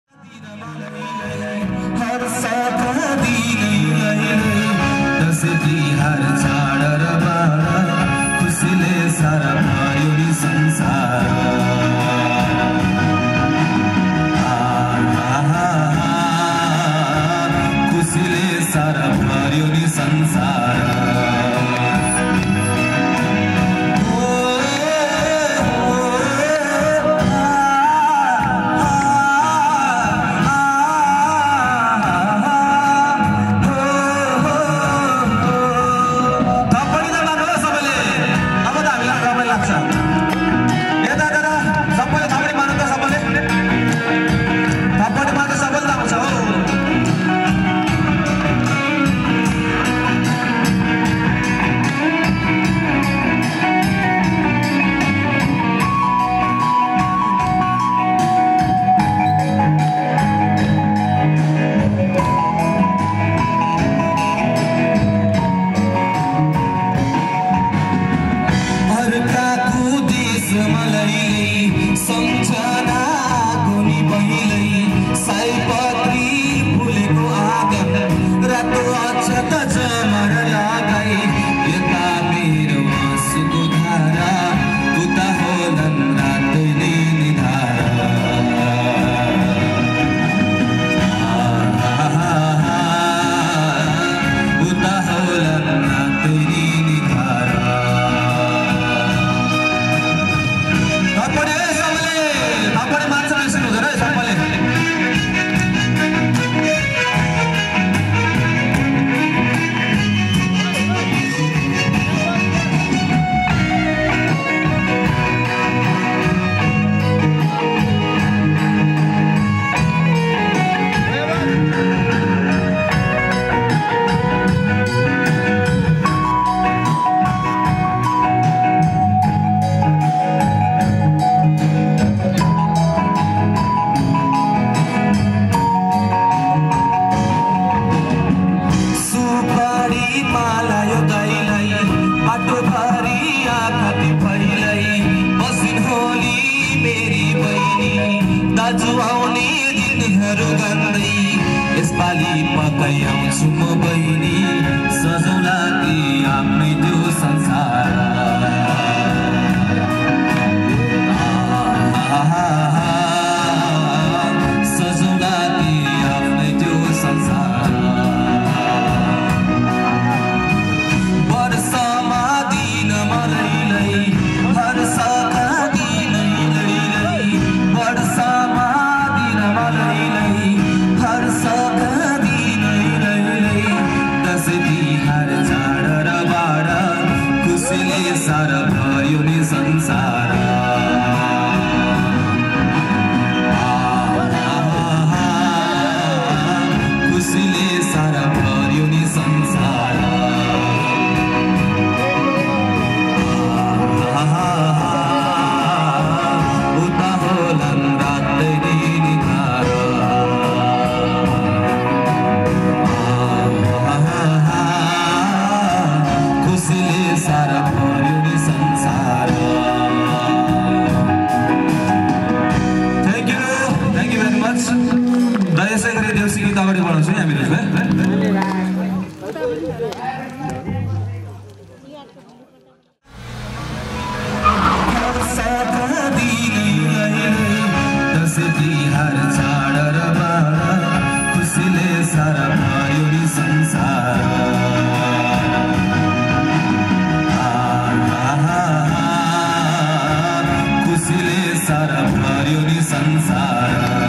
Nepali Tihar Episode - 2 | Deusi Bhailo Song by SASTRA BAND | देउसी भैलो | Dashain Tihar song melody
Singer - Vinod
Lead Guitar - Ashwin
Rhythm - Sahil
Bass Guitar - Aryan
Keyboard - Kushal
Band - ( SASTRA BAND )

Hastags:-

                                       ||   Copyright Disclamer   ||

                                          ||   SONG CREDIT   ||